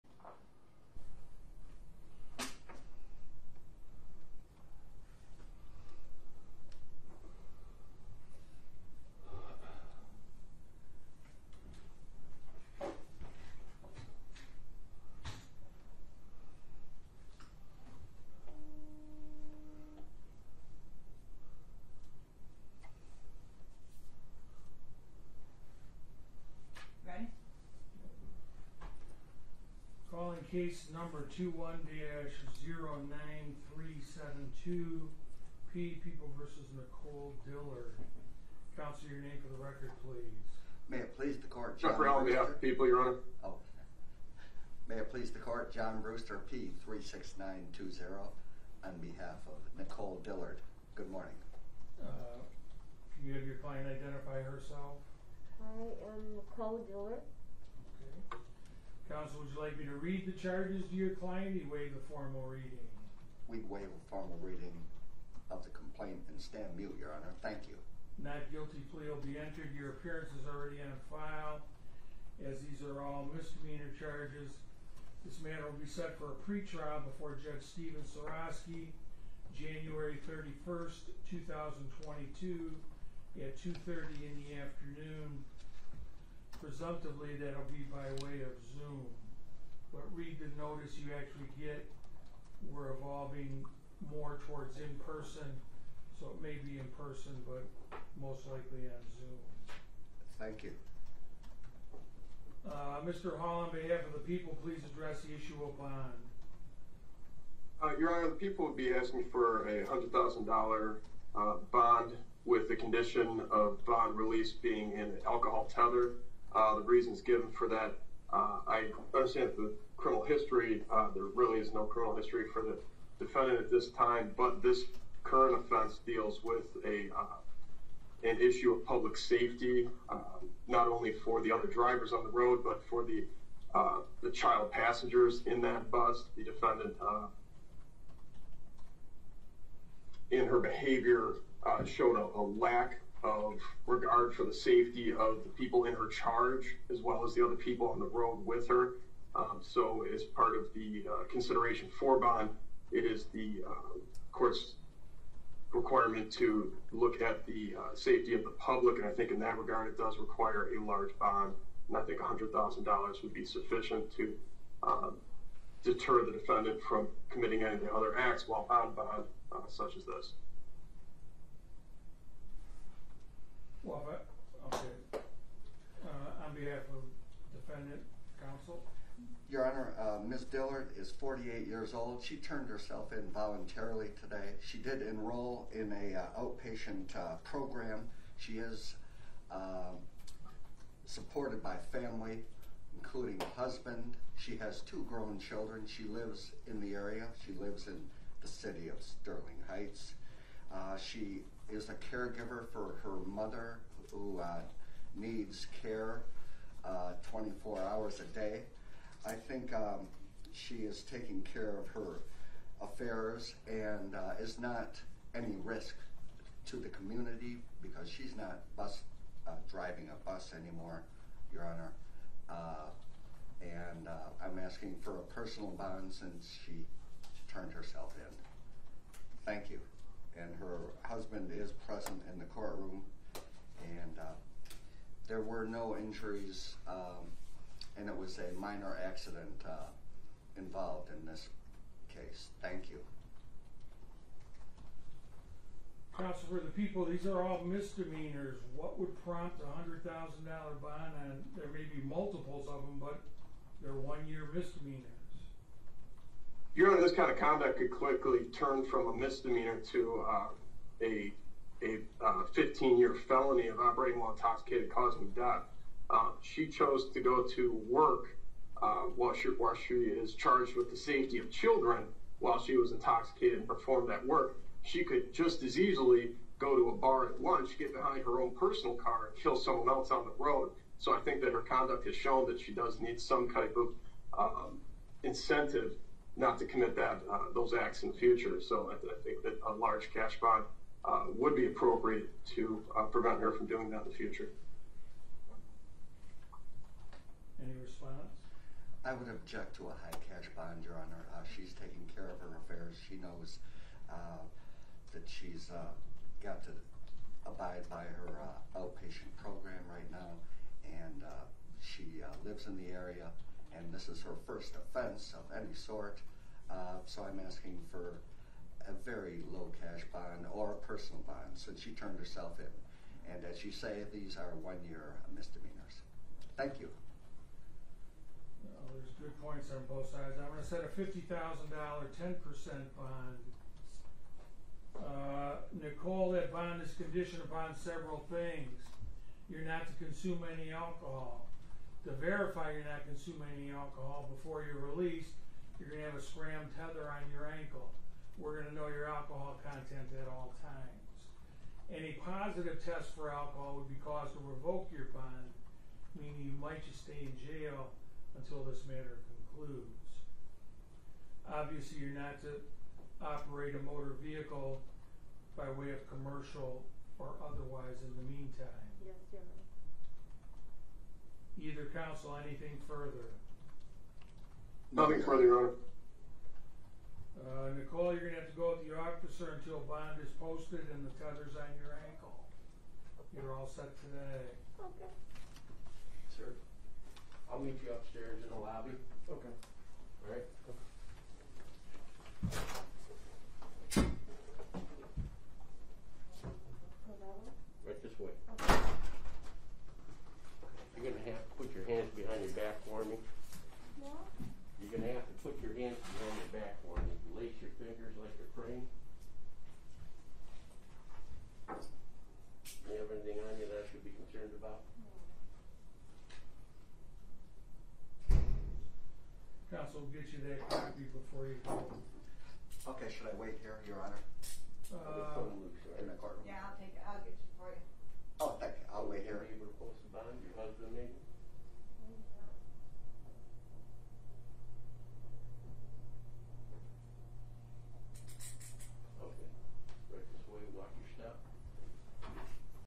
Utica school bus driver arraigned on drunk driving charges
A Utica school bus driver was arraigned on drunk driving charges Monday after her bus crashed with dozens of students onboard.